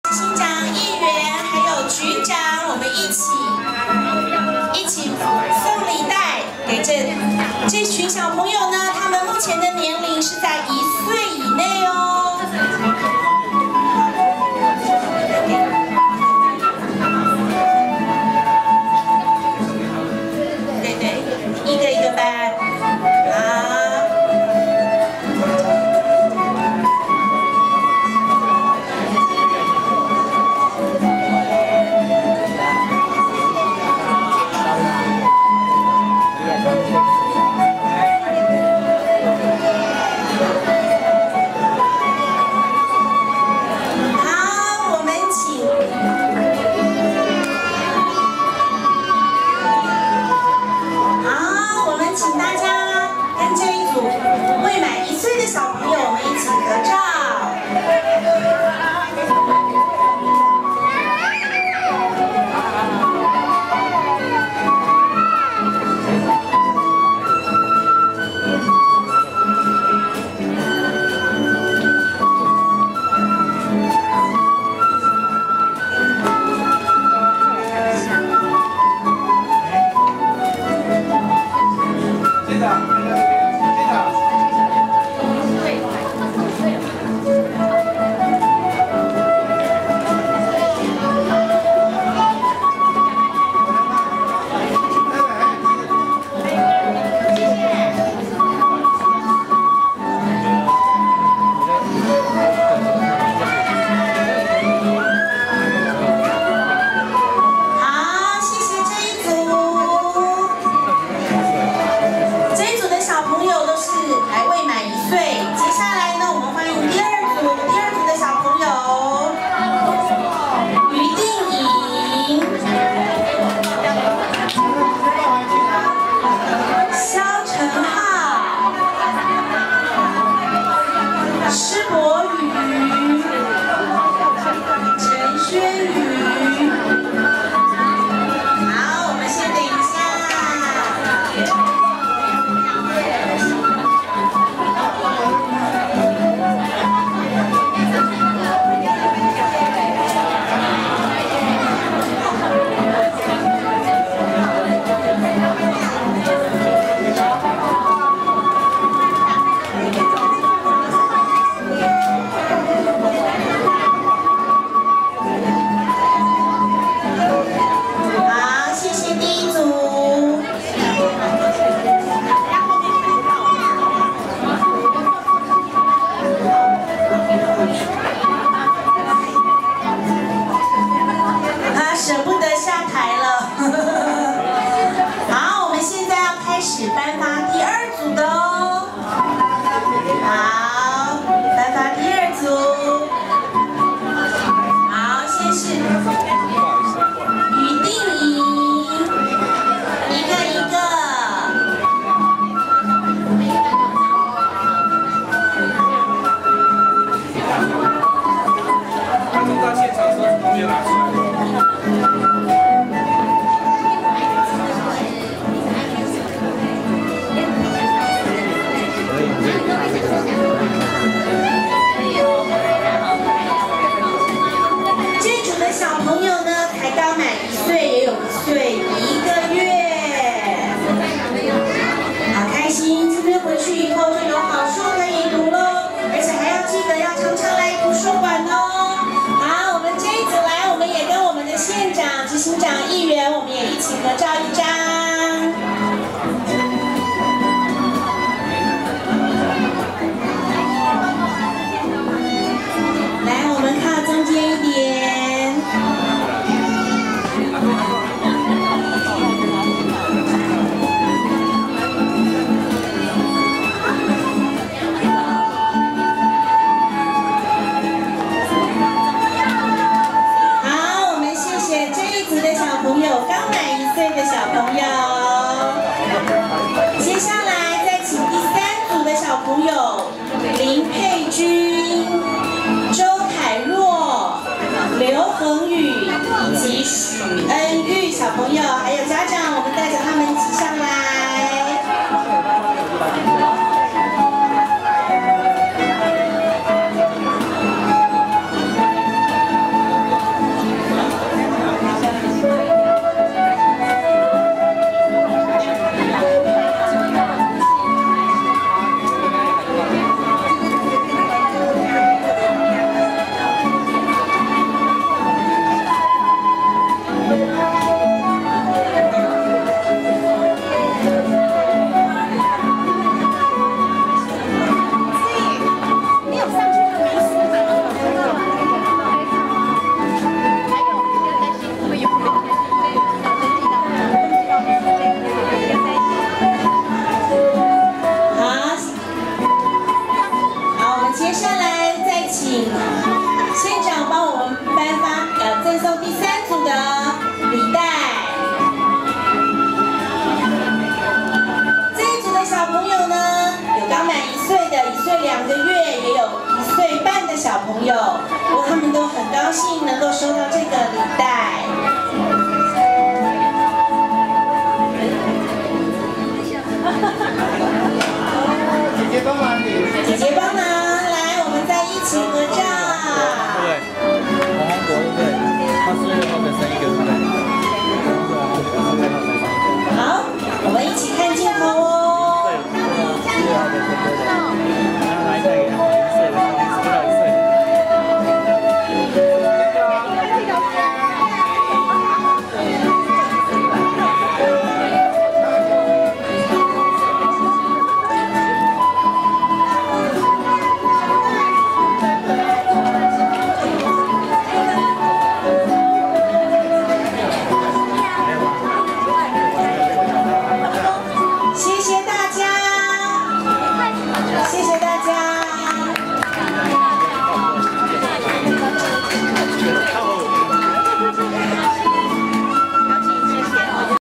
臺北縣立圖書館世界書香日：愛上Bookstart－周錫瑋贈送嬰幼兒閱讀禮袋
周錫瑋縣長親贈閱讀禮袋給本縣年滿6至18個月嬰幼兒，作為本縣送給新生兒的第一份見面禮。